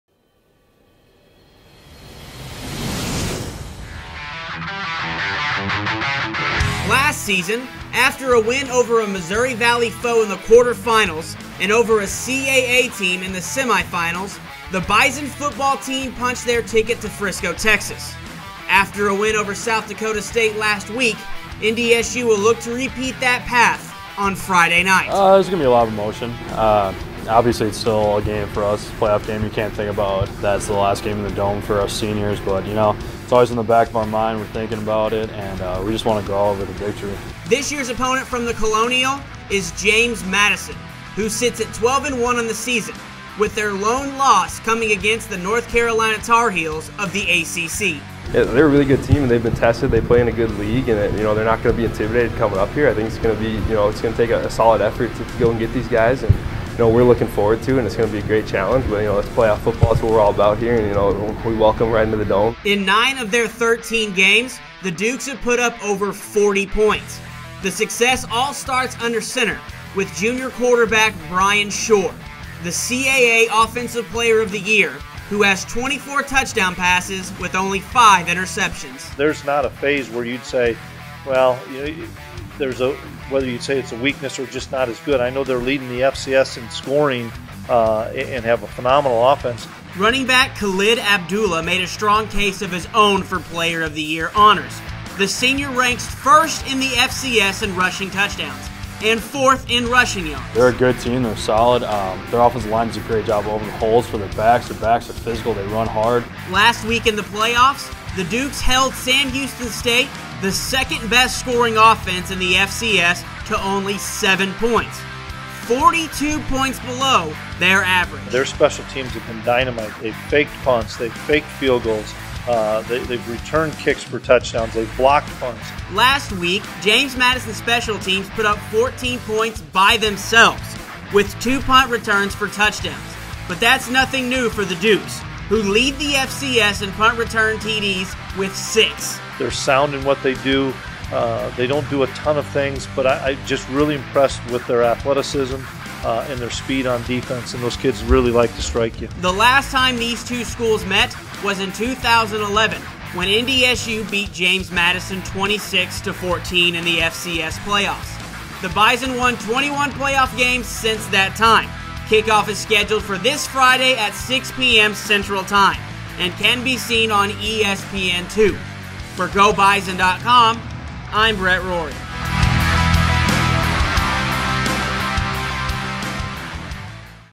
Game Day Preview 2016: James Madison FCS Playoffs Semifinal Round
The North Dakota State Football team will host James Madison University this Friday at the Fargodome.